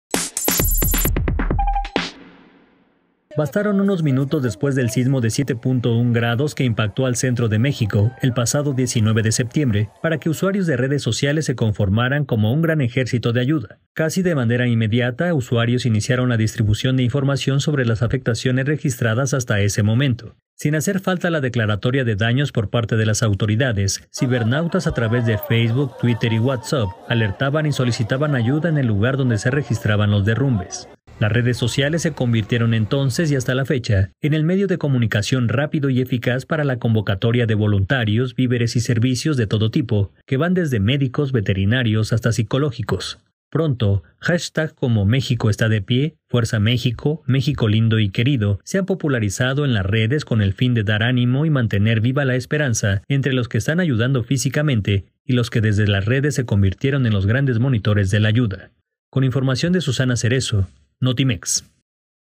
Redes sociales, catalizador de ayuda en México
Usuarios de redes facebook y twitter se convirtieron en un gran ejército de ayuda durante sismo del pasado 19 de septiembre.